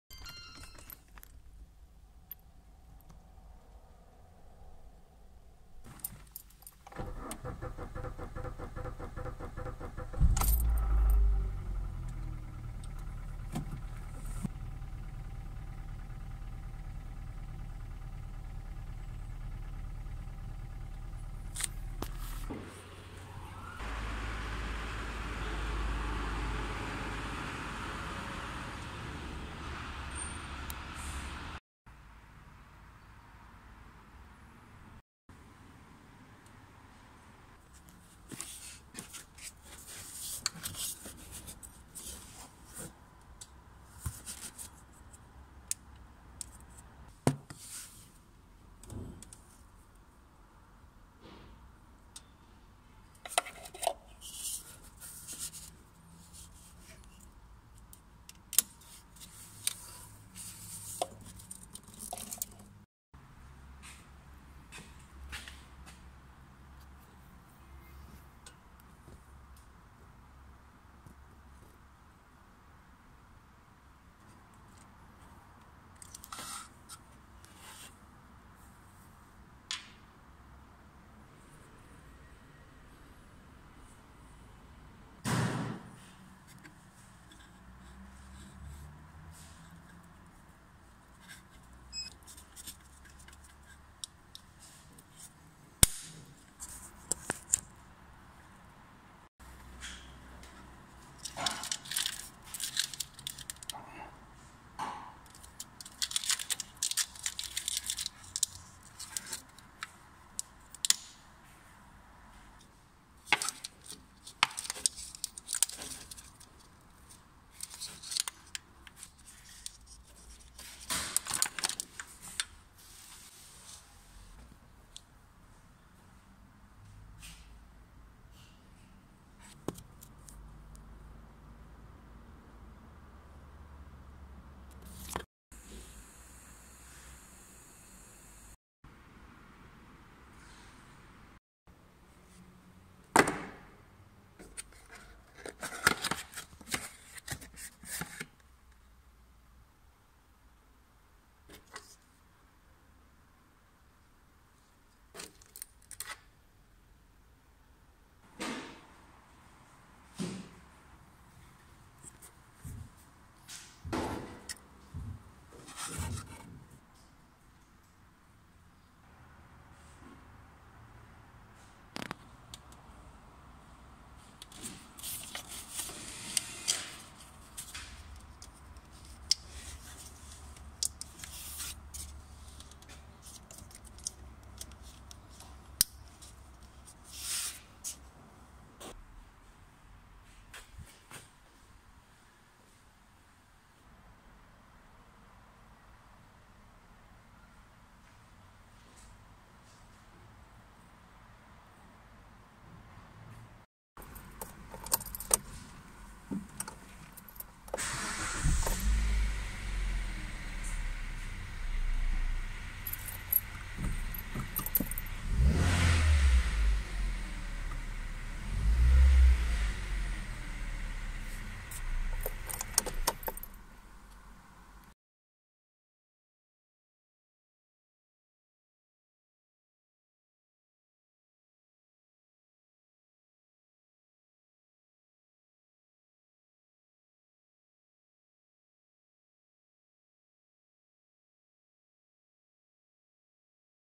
Kia Ceed, Hyundai i30 - crankshaft position sensor, snímač klikové hřídele
Kia Ceed , Hyundai i30

vadný snímač polohy klikové hřídele - výměna
faulty crankshaft position sensor - replacement
defekter Kurbelwellenpositionssensor - Austausch
несправний датчик положення коленвала - заміна
uszkodzony czujnik położenia wału korbowego - wymiana